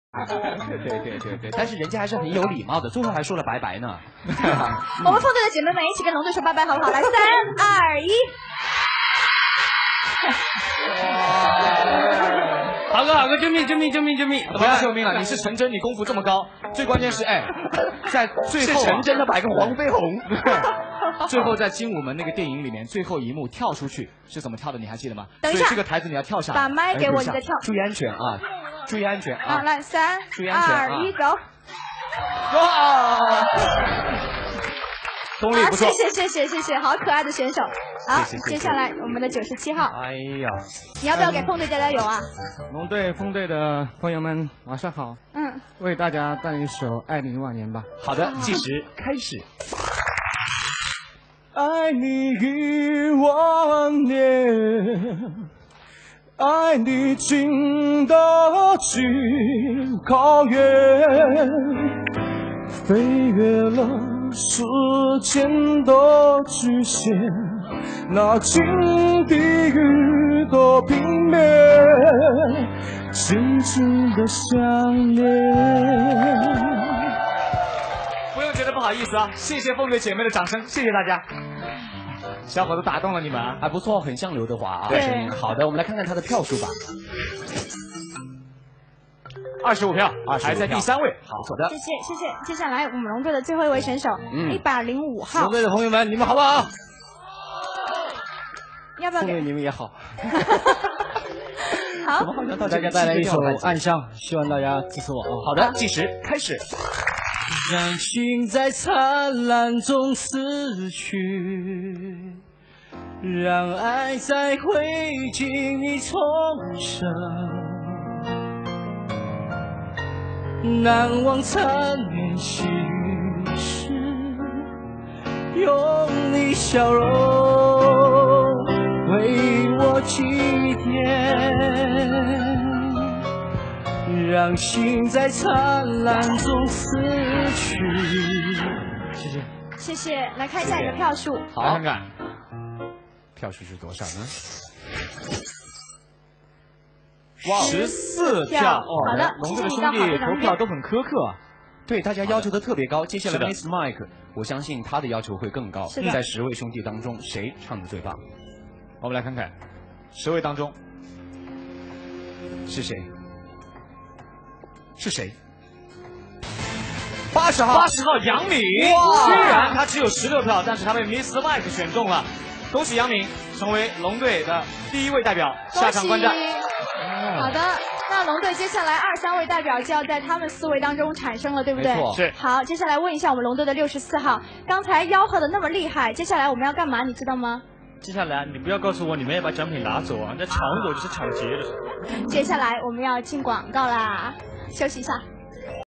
毛毛红衣大跳钢管舞 拿高分三获擂主 - part 13
毛毛红衣大跳钢管舞 拿高分三获擂主